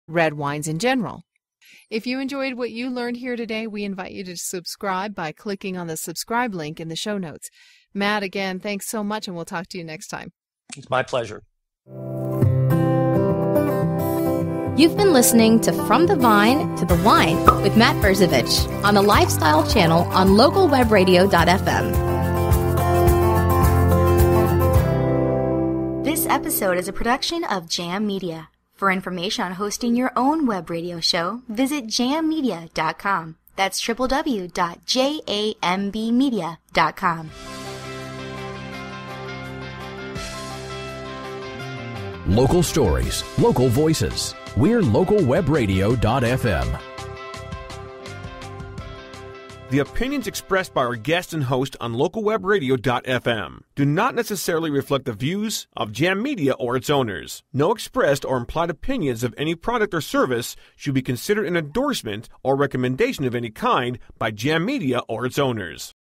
Celebrating Malbec World Day (part 2 of 2, made with Spreaker)
Do you plan to celebrate Malbec World Day? Have you heard of ways to celebrate in the Greater Vancouver area? If you aren't planning to celebrate Malbec World Day, then you're missing out on a rewarding event.

Malbec World Day is an opportunity to experience a bit of Argentina even if you don't live in South America. The event commemorates the day when president Domingo Faustino Sarmiento of Argentina officially made it his mission to transform Argentina's wine industry.

If you're interested in how to select your own bottle of wine for this event, then you'll want to invest 20 minutes to listen to this edition of From the Vine to the Wine where your host Kimberly Henrie interviews local wine expert Matt Brzovic about celebrating Malbec World Day around the globe.

You don't have to be in Argentia to celebrate Malbec World Day. An article in the Vancouver Sun* reports that 40 locations across British Columbia celebrated with free tastings. This article is a chance to learn how to select the best Malbec for your next event.

Why You Need to Know How to Buy the Right Wine

The episode kicks off with a bit of the history about World Malbec Day, including when it started, the person who started it, and where the event originated.

Choosing a bottle of wine can be daunting, especially if you don't know about the grapes used to create the wines. During the podcast Matt talks about both entry-level wines and ones that you should store in your cellar until a special occasion.

If you're not aware of how to choose the right bottle of wine, then you could be wasting money on the wrong types.

How to Select the Right Wine for Your Budget

Next Matt discusses some history behind the Malbec grapes, and talks about how altitude affects the quality of grapes in reference to wine making. During this episode he also answers these questions:

- What flavor profile can I expect from Malbec wines?
- Does all Malbec taste the same depending on the price?
- Why is Malbec so popular on the Semiahmoo Peninsula?

Now ask yourself two questions. Do you do your homework before buying wine? Have you enjoyed the flavor of Malbec? If you answered "No" to either or both of these questions it's time to introduce new inventory into your wine collection.

Start Rethinking Your Rules and Relationships

Matt goes on to explain the variations in price for different wines. He shares the price range that he finds most comfortable, but also offers some alternatives as well as tips to help you decide which is best for you.

There's a difference between everyday wine and more serious products. Depending on your taste and budget, these wines could come from anywhere around the world. Wine connoisseurs who research are able to obtain new bottles to satisfy both flavor preference and affordability.

Knowing where to buy wine is the next step in selecting ones that offer the best taste for the value. Local wine experts can help you find just what you're looking for. Why aren't you asking them for advice?.

The episode wraps up with Matt describing this week's Wines of the Week, including the Malbec blends. He shares about two wines from slightly different ends of the spectrum. You won't want to miss his tips.

Are You Ready to Add to Your Wine Collection?

Which Malbec do you plan to add to your wine cellar? Could you use advice before shopping for new wines? Listen to the entire episode now. Then if you have questions or wine suggestions of your own to share, please comment in the space below so we can discuss those with you.